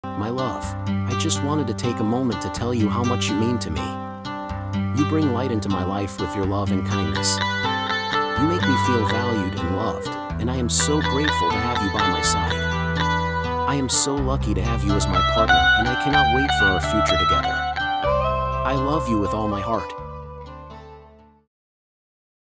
How to express love :Unconditionallove
this video is about Eternal Love: A Video Tribute to Unending Devotion, created by DVens Creation @shillong